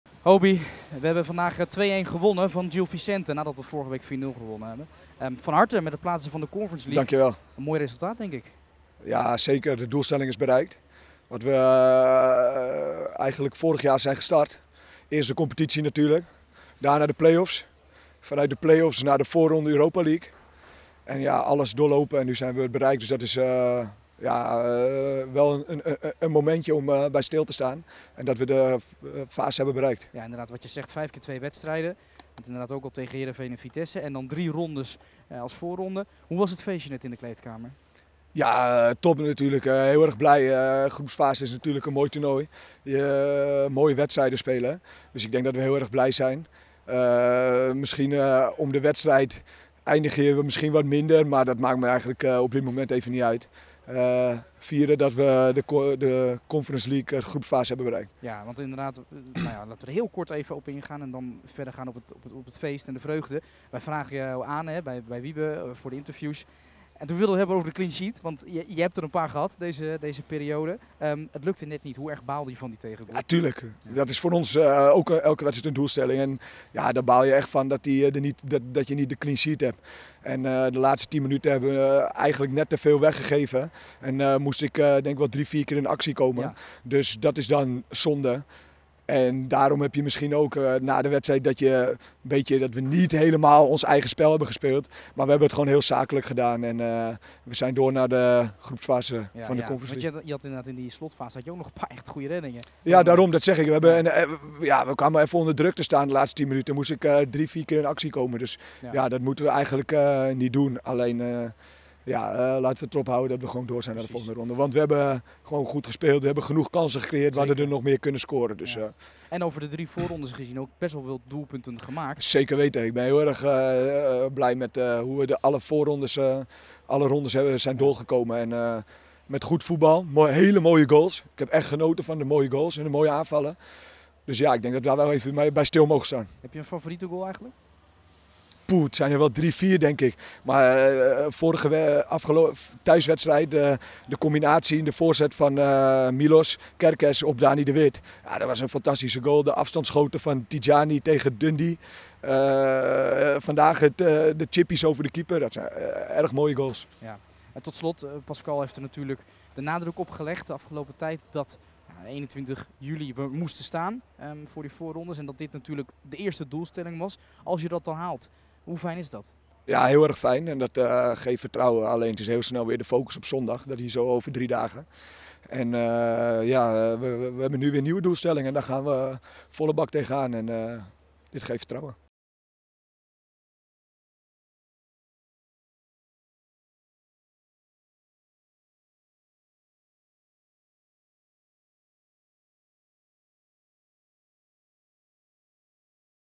💼 Verhulst: '𝗭𝗮𝗸𝗲𝗹𝗶𝗷𝗸 𝗴𝗲𝗱𝗮𝗮𝗻' | Gil Vicente FC - AZ
Hobie Verhulst stond na afloop van het Conference League-duel met Gil Vicente voor de camera van AZ TV. Over de late tegengoal, doelstellingen en het duel in Barcelos.